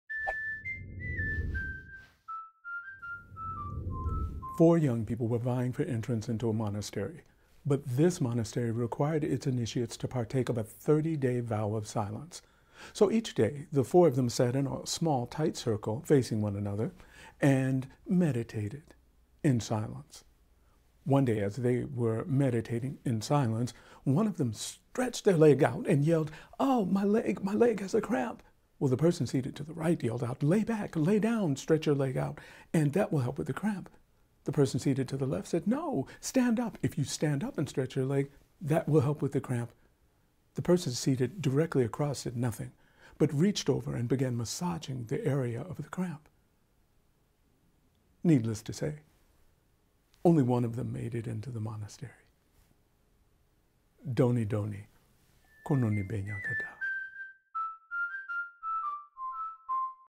Vows of Silence - A Tale of Wisdom with Baba the Storyteller
In this video, Baba the Storyteller shares a Tale of Wisdom titled Vows of Silence in which there are consequences for actions.